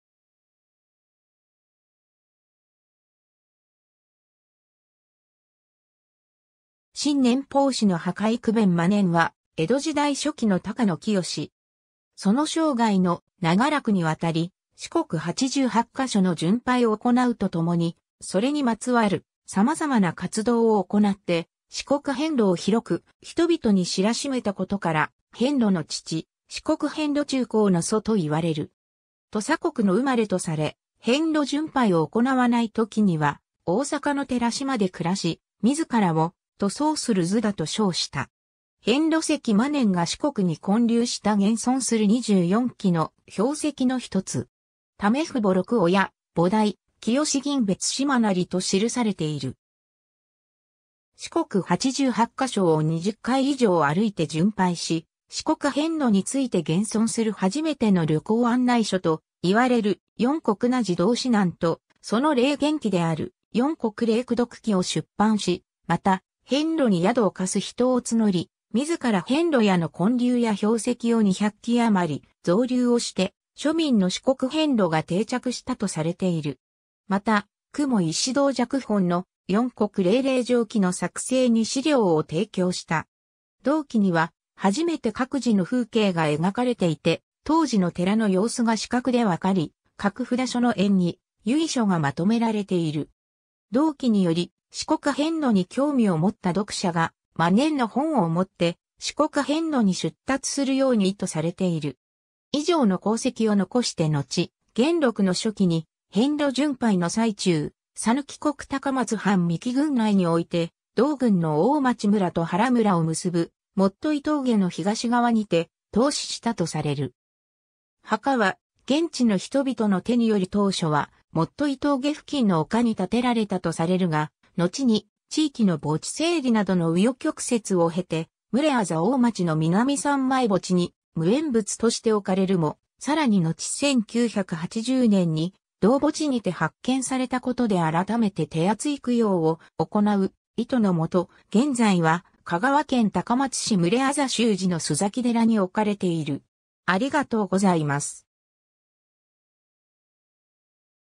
真念
真念法師の墓（洲崎寺） 宥辨 真念（ゆうべん しんねん、出生年は不明、没年は1692年〈元禄5年〉、または1691年〈元禄4年〉の説もある）は、江戸時代初期の高野聖。
その生涯の長らくにわたり四国八十八箇所の巡拝を行うとともに、それにまつわる様々な活動を行って四国遍路を広く人々に知らしめたことから「遍路の父」「四国遍路中興の祖」と云われる。
土佐国の生まれとされ、遍路巡拝を行わない時には大阪の寺嶋で暮らし、自らを抖そうする頭陀と称した。
 遍路石（延命寺） 真念が四国に建立した現存する24基の標石のひとつ。
「為父母六親菩提澄吟 別島也」と記されている。
 四国八十八箇所を20回以上歩いて巡拝し、四国遍路について現存する初めての旅行案内書と云われる『四國邊路道指南（しこくへんろみちしるべ）』と、その霊験記である『四國徧禮功徳記』を出版し、また、遍路に宿を貸す人を募り、自ら遍路屋（真念庵）の建立や標石を200基余造立をして、庶民の四国遍路が定着したとされている。
 また、雲石堂寂本の『四國徧禮霊場記』の作成に資料を提供した。
同記には、初めて各寺の風景が描かれていて、当時の寺の様子が視覚でわかり、各札所の縁起・由緒がまとめられている。
同記により四国遍路に興味を持った読者が、真念の本を持って四国遍路に出立するように意図されている。
 以上の功績を遺してのち、元禄の初期に遍路巡拝の最中、讃岐国高松藩三木郡内において同郡の大町村と原村（現在の香川県高松市牟礼字大町から字原の区間）を結ぶ元結（もといむすび）峠の東側（原村側）にて倒死したとされる。
墓は現地の人々の手により当初は元結峠付近の丘に建てられたとされるが、のちに地域の墓地整理などの紆余曲折を経て牟礼字大町の南三昧墓地に無縁仏として置かれるも、さらにのち1980年（昭和55年）に同墓地にて発見されたことで改めて手厚い供養を行う意図の元、現在は香川県高松市牟礼字宗時の洲崎寺に置かれている。